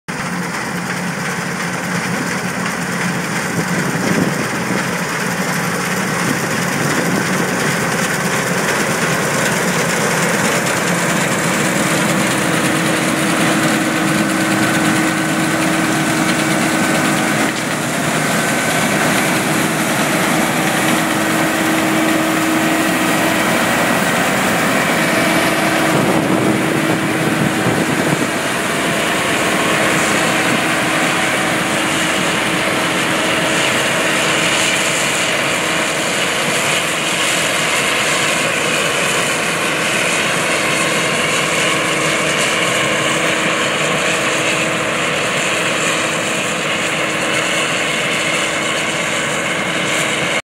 Newholland TF 46
Harvesting winter barley in Kilkenny